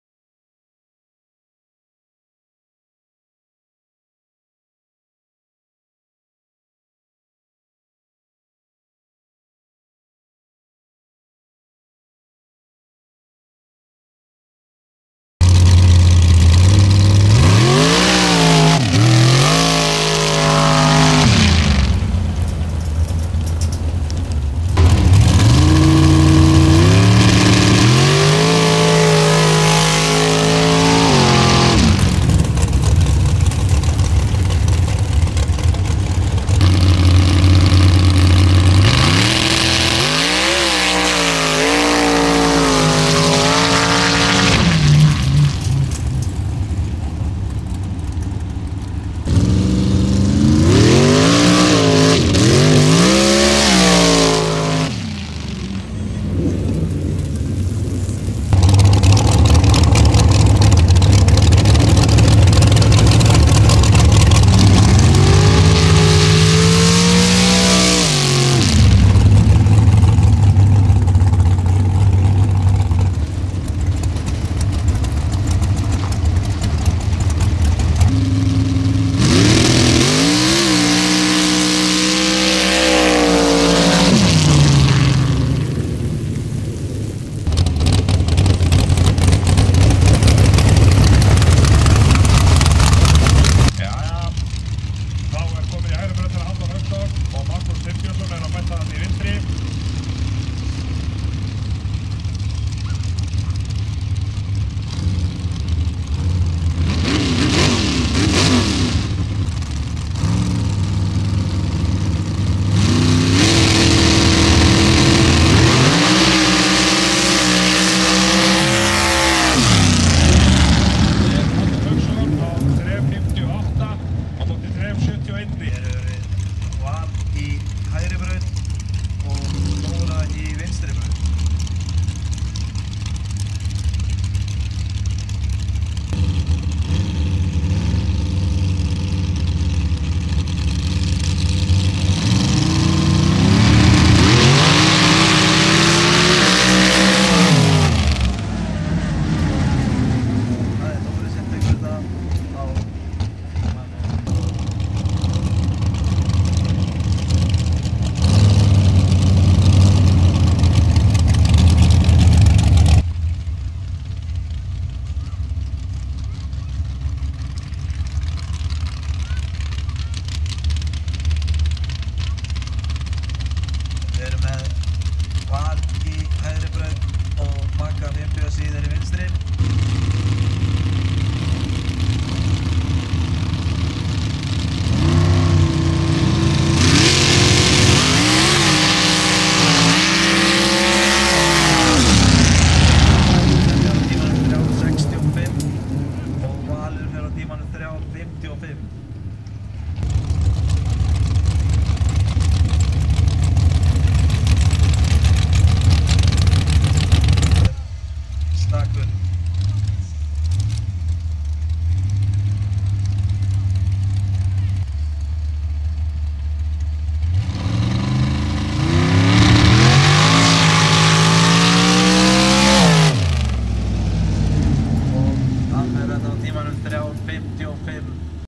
Sand Drag Racing Iceland 2018! 2.round OF
Sand Drag Racing Iceland 2018!
2.round in Icelandic Championship. OF Class.